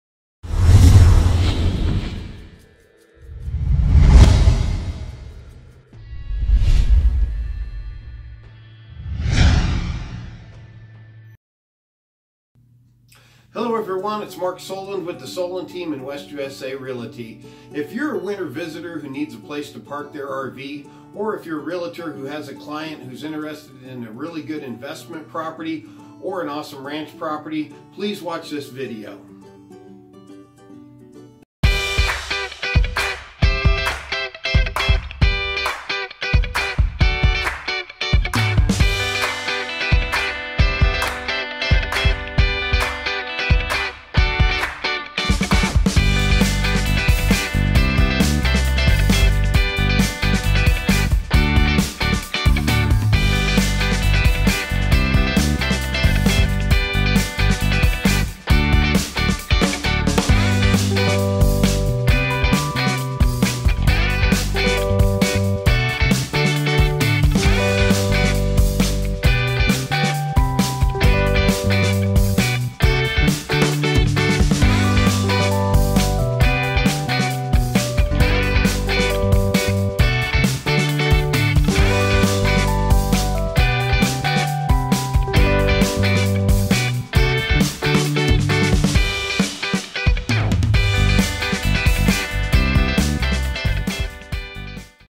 605 N. 347TH AVE. Tonopah, AZ
An Awesome 1 acre property with a wide open floorplan, an oversized garage, a RV carport, Solar panels, and mush much more!!!